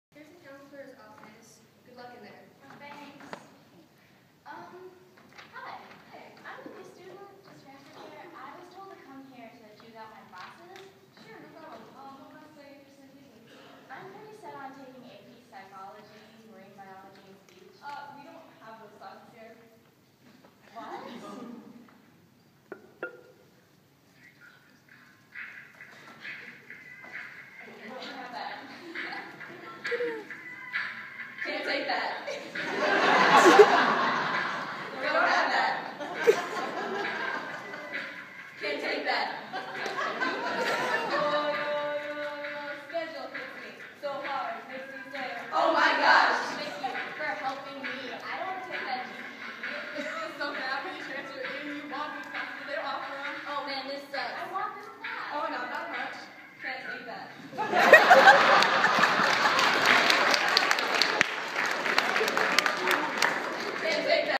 Teen AFAP- U Can't Take That!
Kaiserslautern Armed Forces Action Plan Teen Workgroup skit!  Saige is wearing the white jacket with the sweet dance moves!!